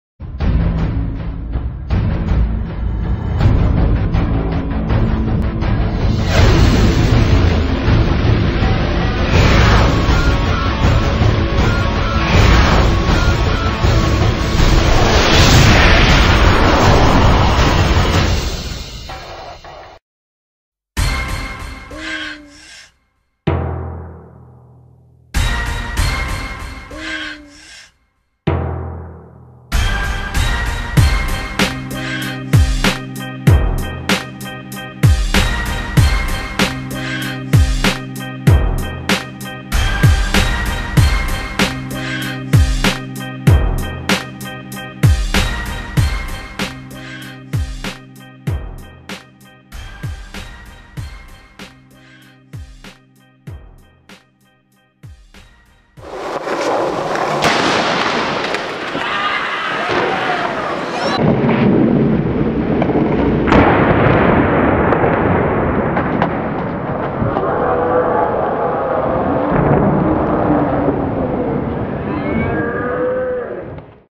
[LSD SKATE SISTEM] TRAILER SUMMER VIDEO 2010
Pequeño adelanto de
LISIADOS
SUMMER
2010
EDICION: MARKLEI MARTIN
POST EDICION: MARKLEI MARTIN
Filmado por: MARKLEI & FACUNDO

En el video: [David Sanchez, Ricard Forcadell, Marklei Martin, Xavi Anguita, Sergio Garcia, Facundo Altamirano, Sergi, Jassin Madih]